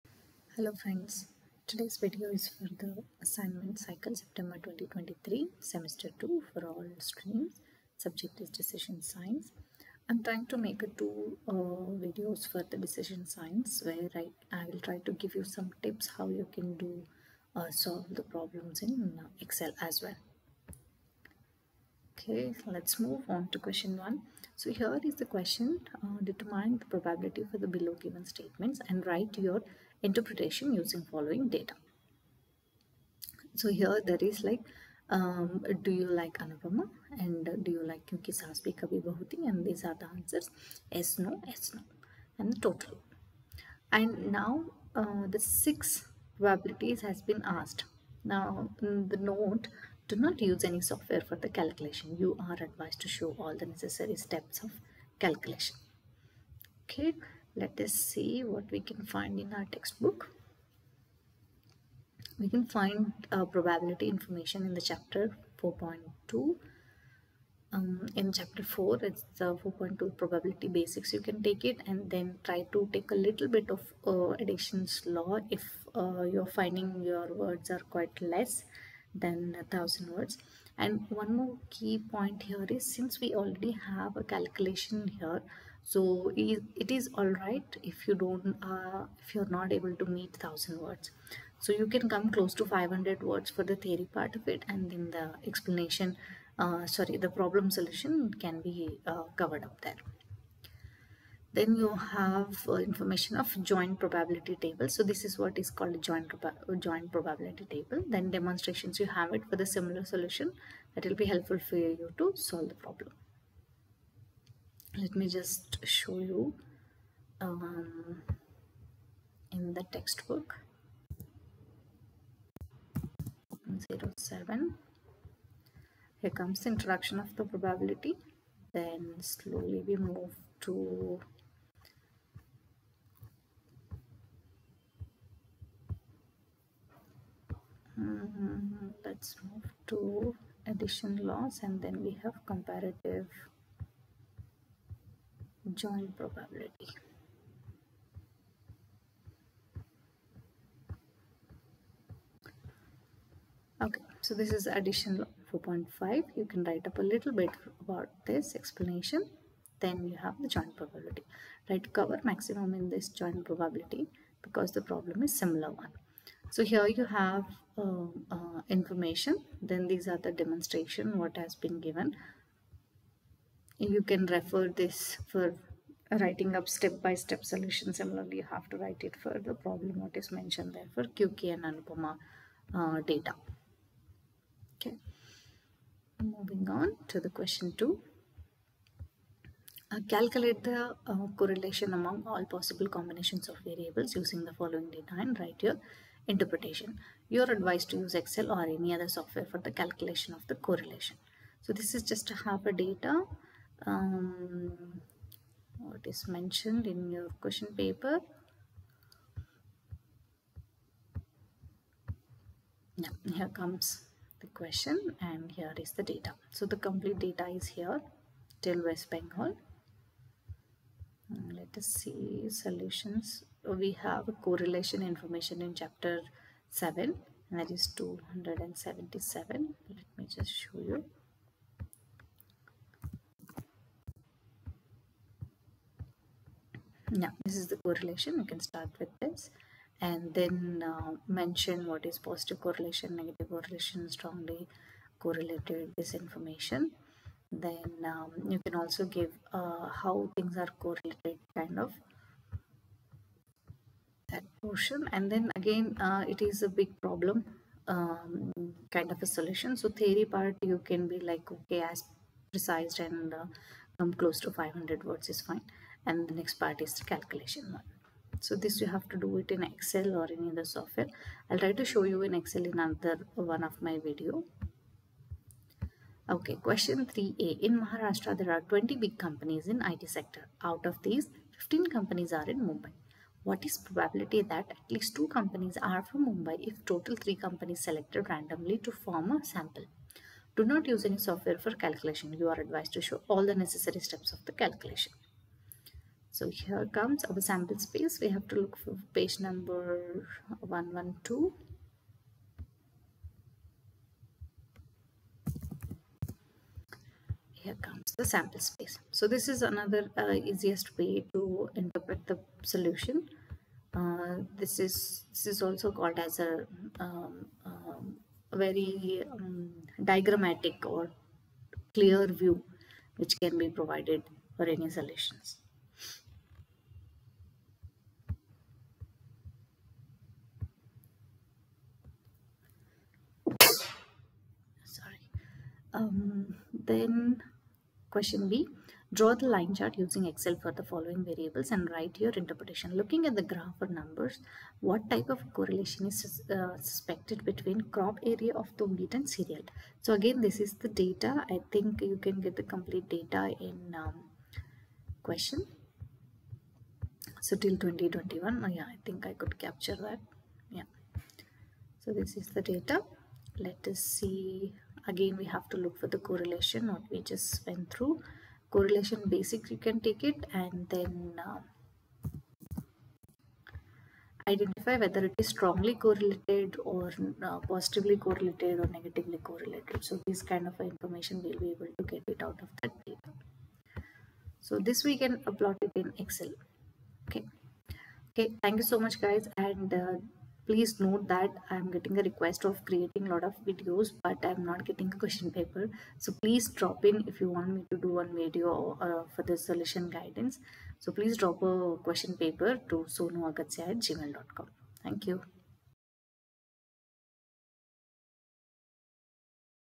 NMIMS -Sept 2023 Assignment-Decision Science(MS excell) : SEM2_All
This video will provide guidance to write the assignment for the NMIMS Distance MBA - Sept 2023